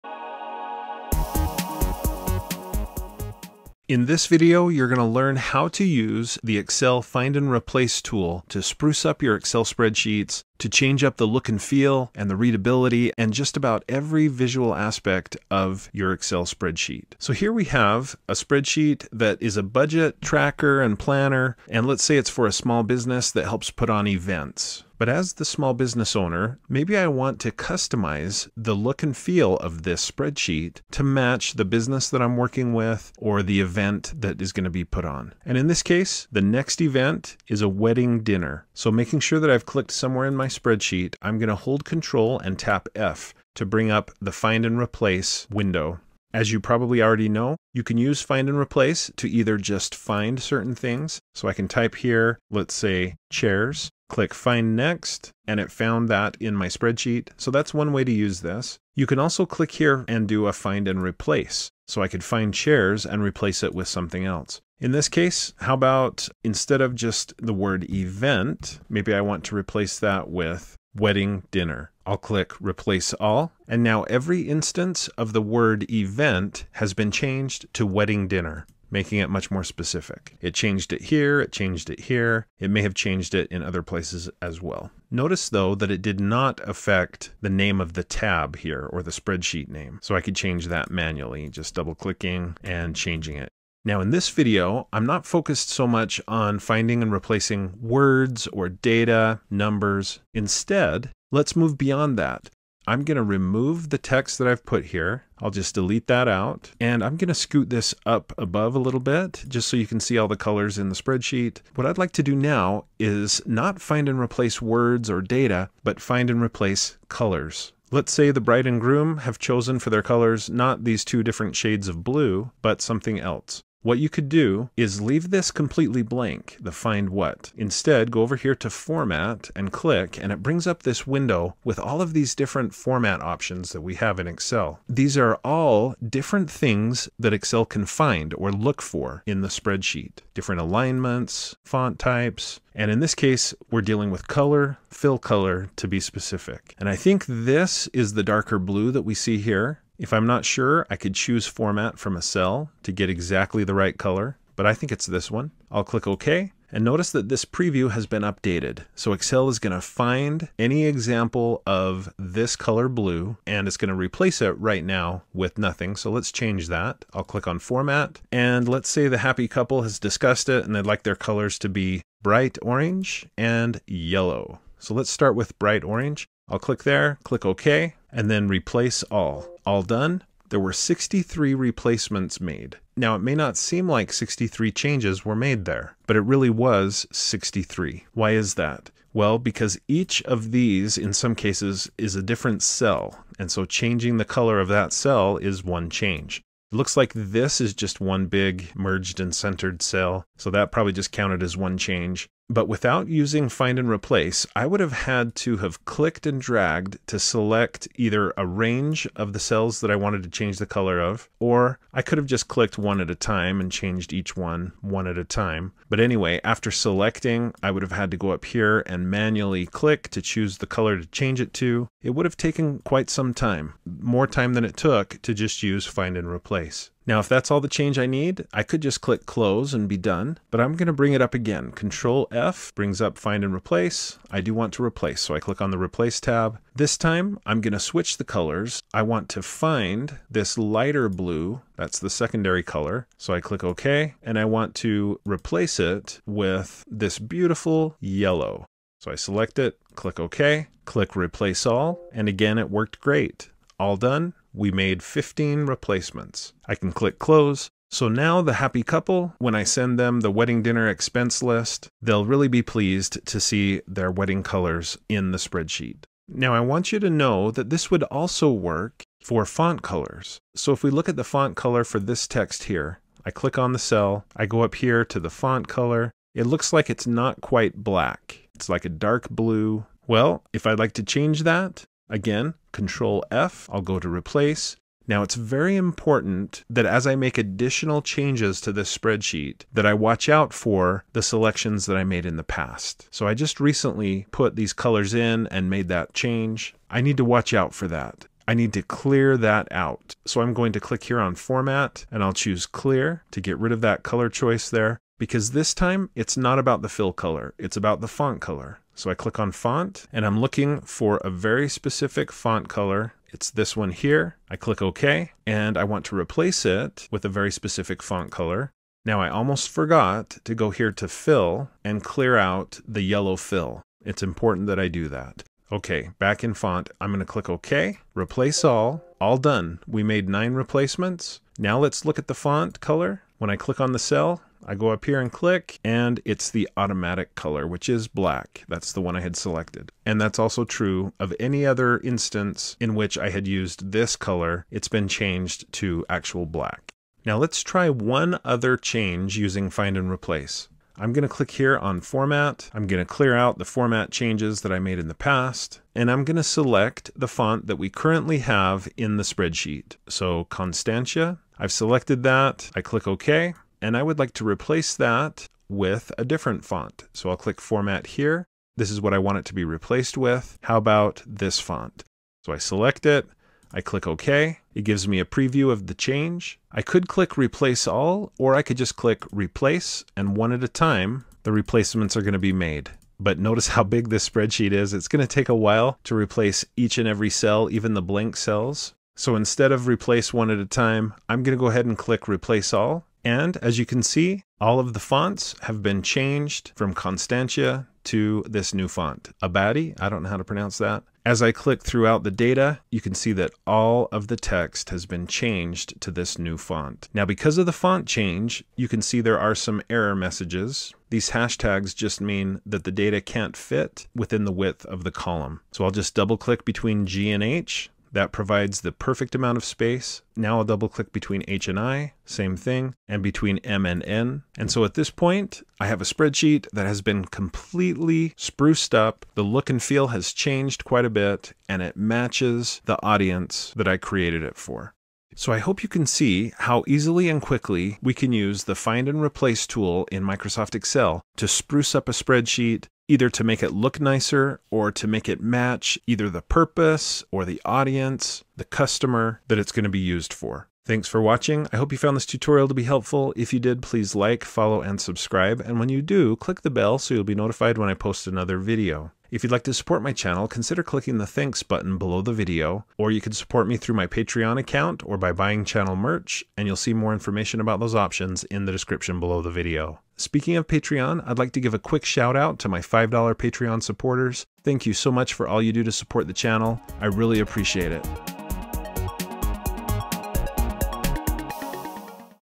Spruce up Your Excel Spreadsheets with Find and Replace
Learn how to use Find and Replace in Excel to do MUCH more than just replace text. Find and Replace makes it easy to change the look and feel of your spreadsheets by instantly changing cell and text color, font type, and much more. Here's a link to the practice file for this video